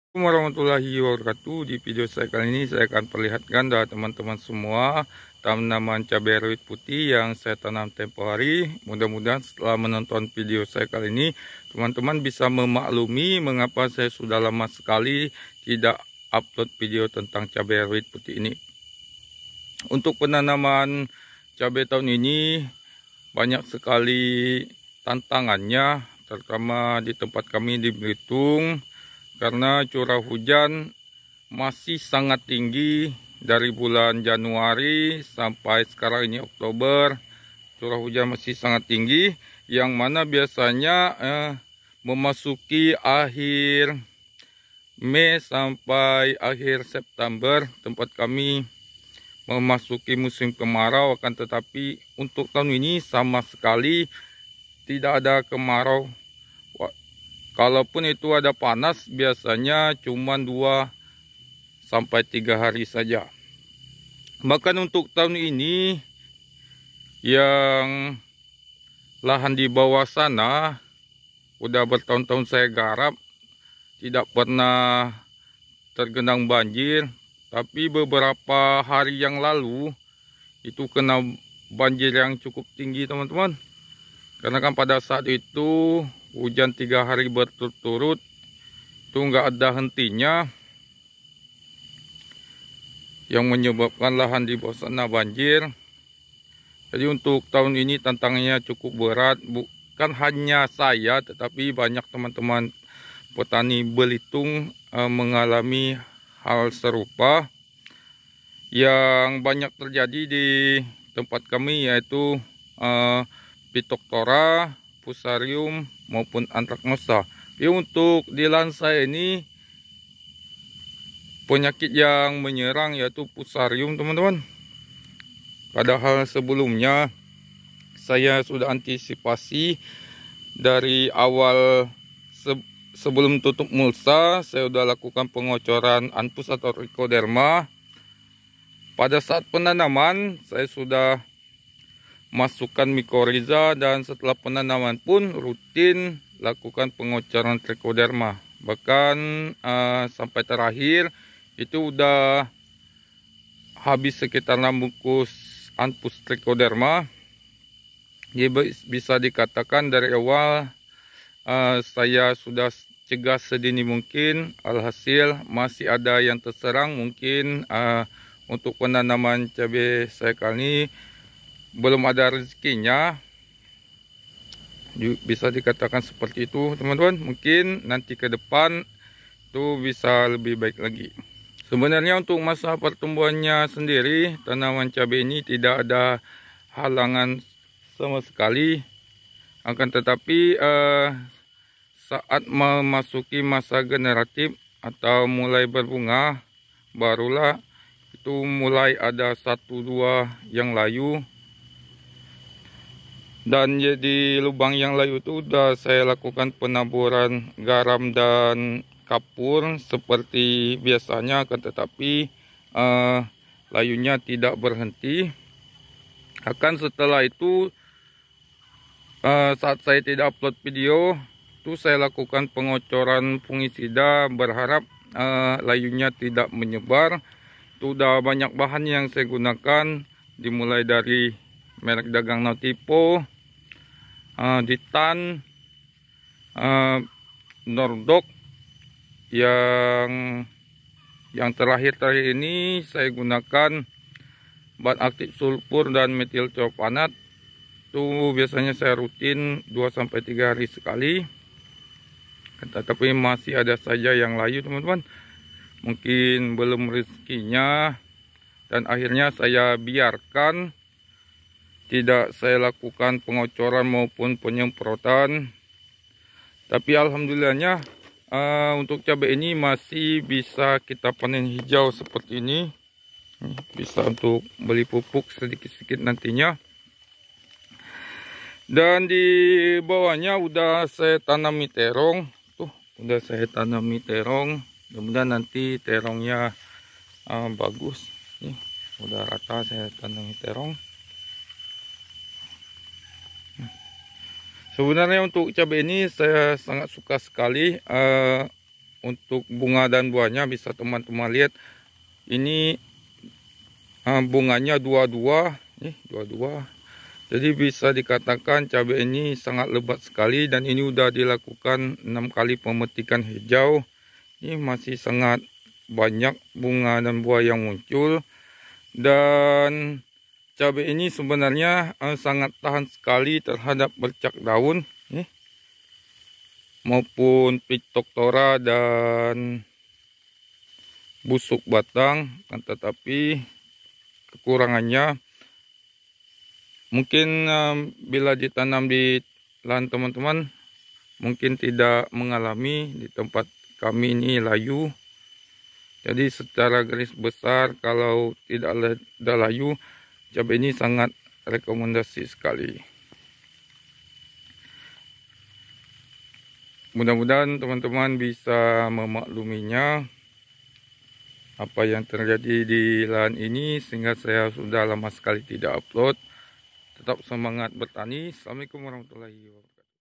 Mohon maaf sebesar besar nya
Di video kali ini saya akan jelaskan pada teman-teman semua mengapa saya sudah lama tidak upload  video cabe rawit